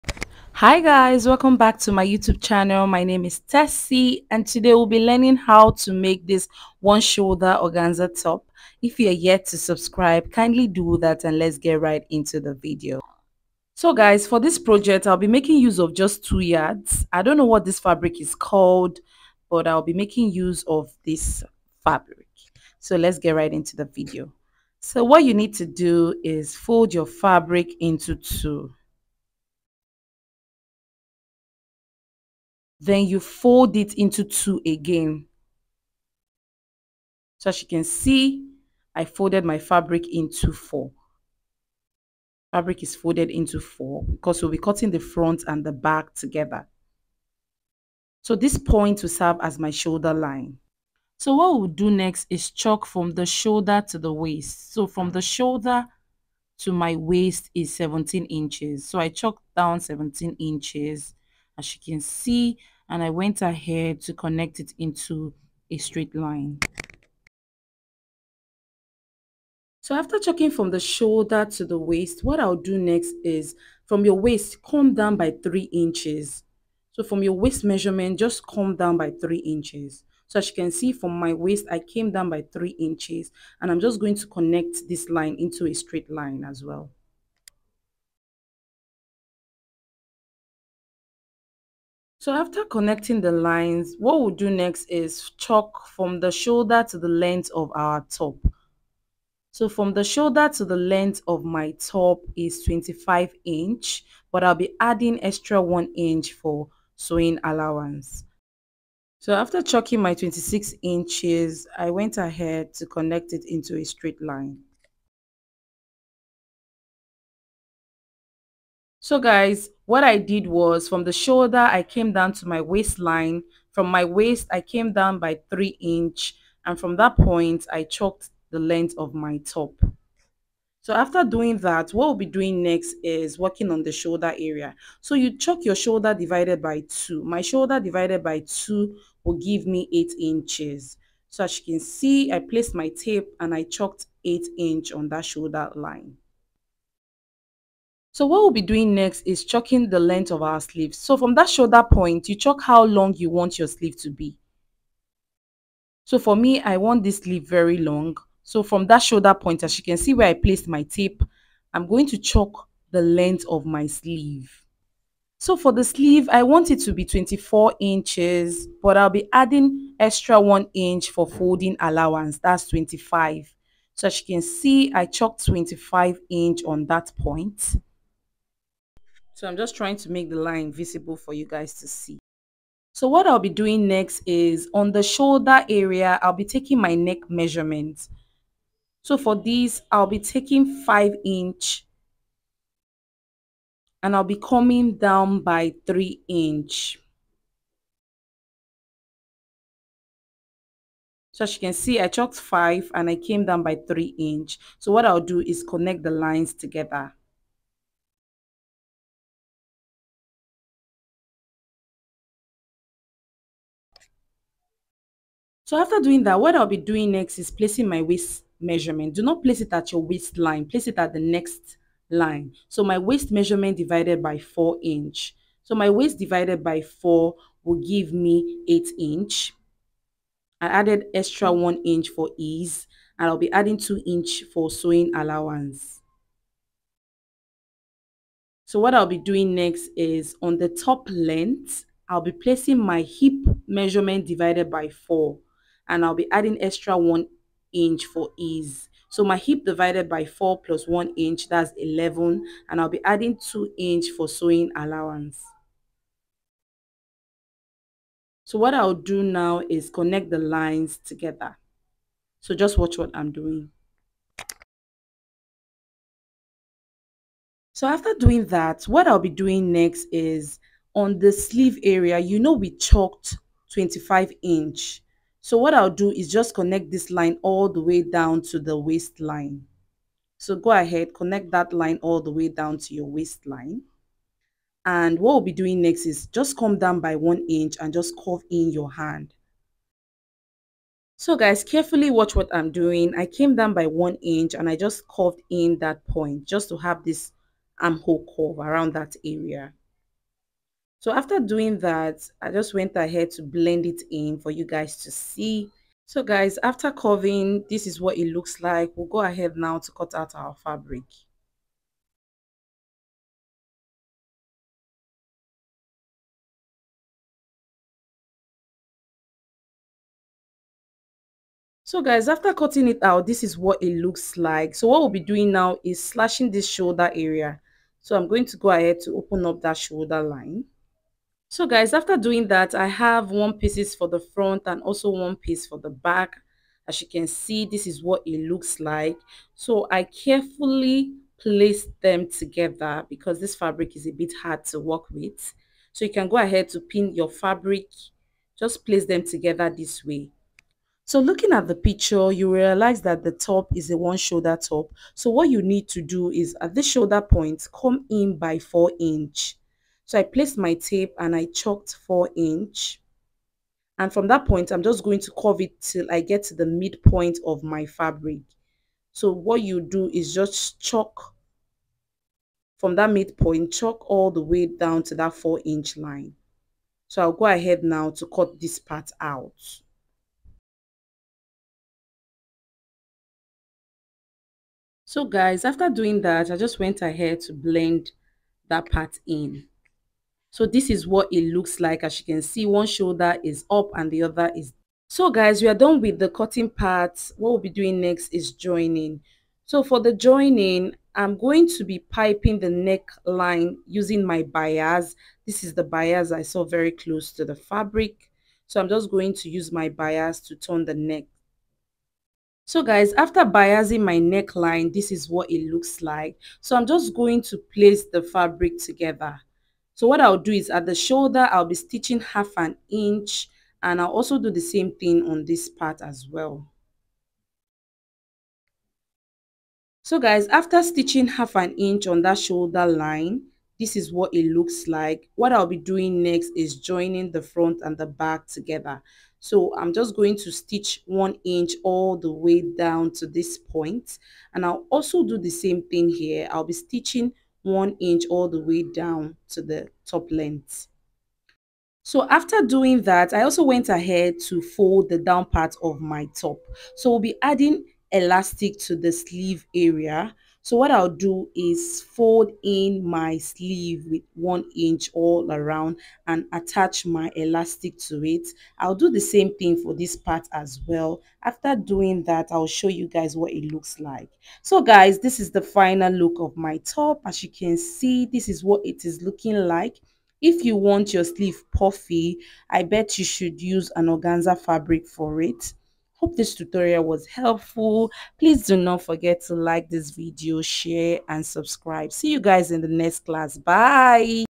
How To Make An Organza One Shoulder Top
This is a very detailed tutorial on how to make an organza one shoulder top …..